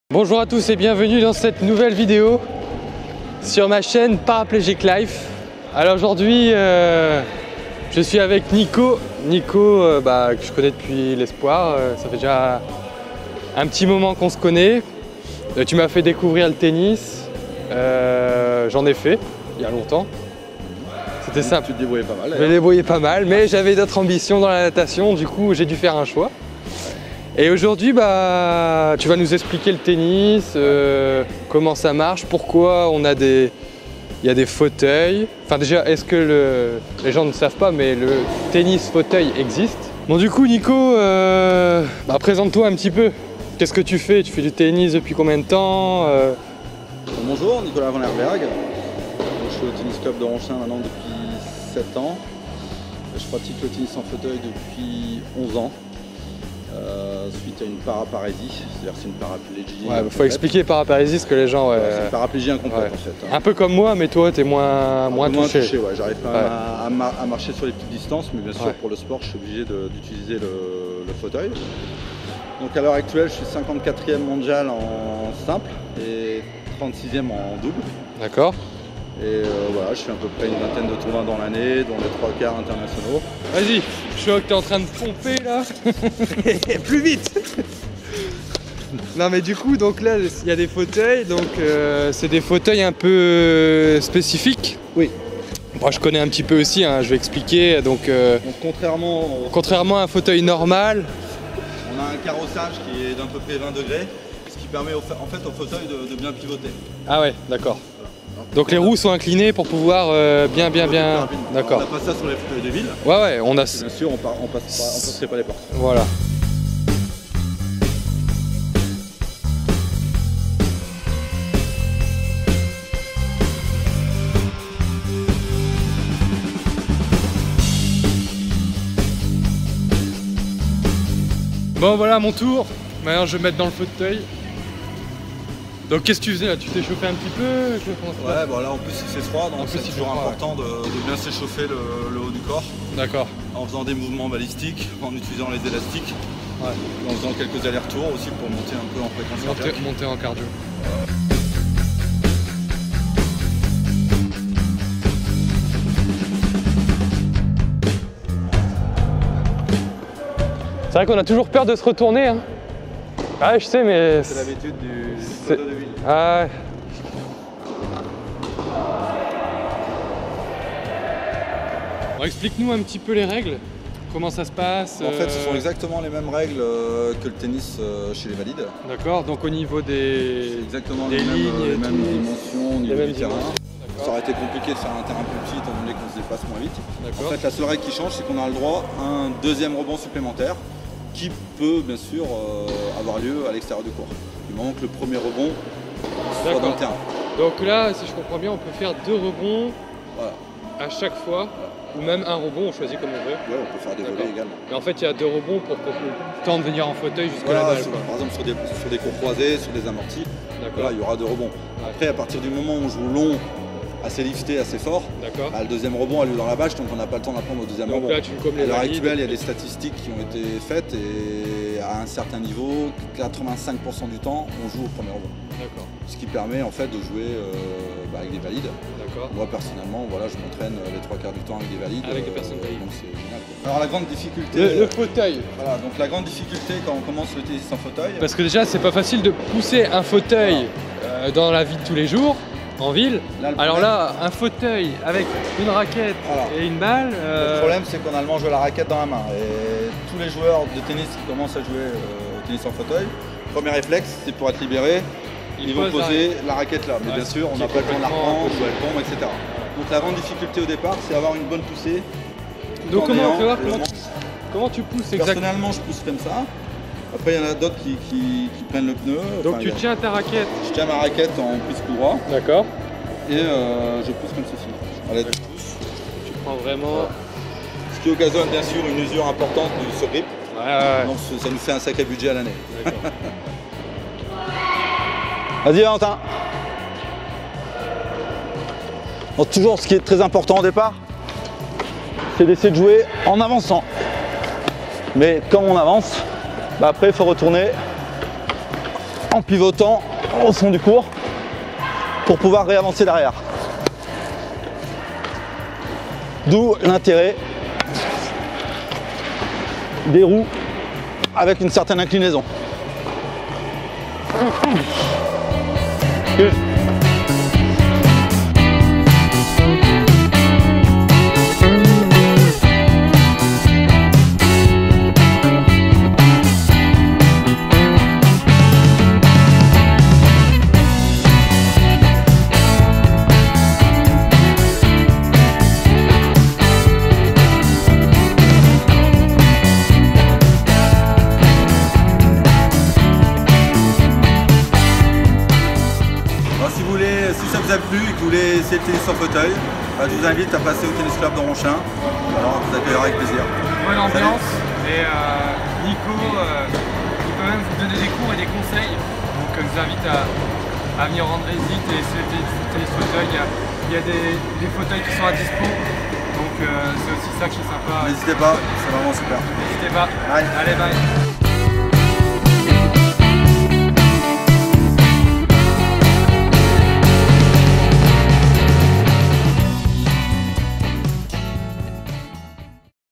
Tennis en fauteuil roulant !
Me voilà dans un fauteuil roulant pour jouer au tennis. J'ai eu la chance de pouvoir tester le tennis fauteuil en compagnie du numéro 5 Français.
Moi qui pensais que le tennis n'était plus accessible pour les personnes en fauteuil...
Après plusieurs conseils, j'ai vite pris la main sur la gestion du fauteuil roulant et la manipulation de la raquette. Dans cette vidéo, on vous donne quelques astuces pour commencer le tennis fauteuil, on vous explique les règles et on vous fait une petite démonstration.
 Je conseille cette vidéo aux personnes qui souhaitent se remettre au sport après accident (parplégie, etc..).

Laissez-vos commentaire plus bas.
TeamAA sport

► ► ► ►Qui suis-je ?